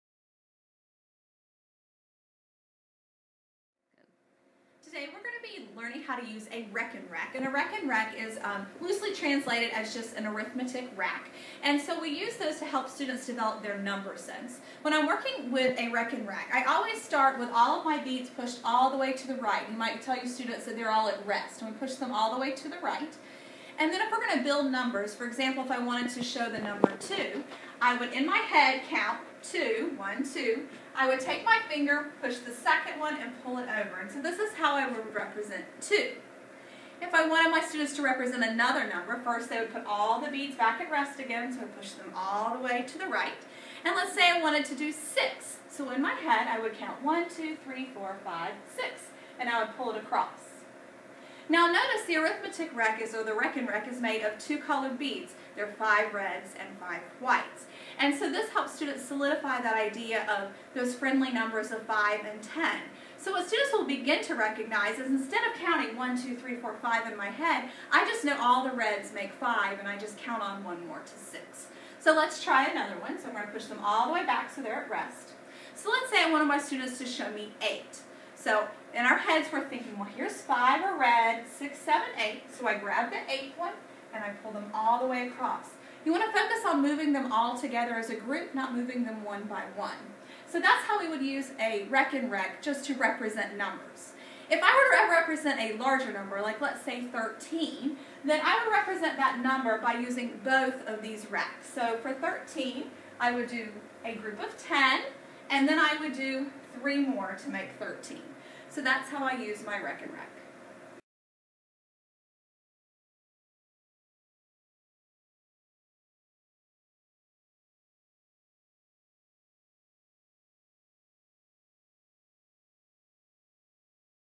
How to Use a Rekenrek
This video provides an overview of rekenreks, a math tool that can be used for representing numbers, subitzing, conservation of numbers, and decomposing numbers.  This video will be helpful to watch before doing the Cumberland County "building fluency" numbers talks in kindergarten.